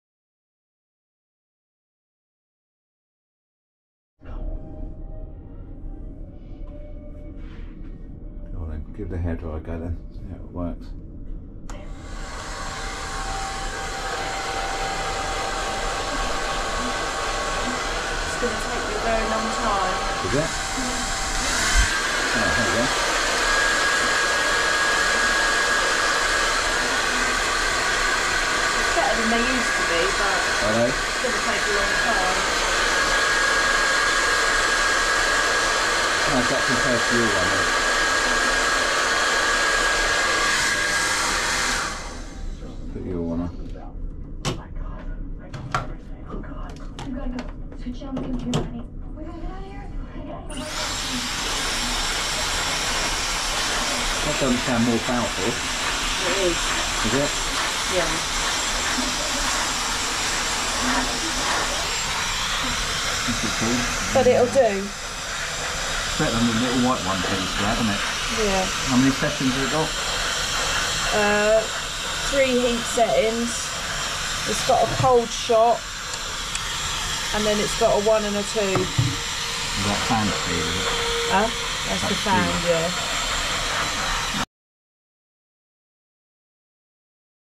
P&O Arvia Cruise Ship cabin hairdryer Valera Swiss Light
Probably our most important cruise ship video! Kaz checks out the in-cabin haridryer on P&O Arvia. How does it compare to her Dyson?

I would have given it a try but my lack of hair might not have been a fair test.

P&O have invested in a Valera Swiss Light 3200 Professional hair dryer. You can buy these online at around £48 each. It has a 1600 W motor, a cool air button and 6 air flow/temperature settings.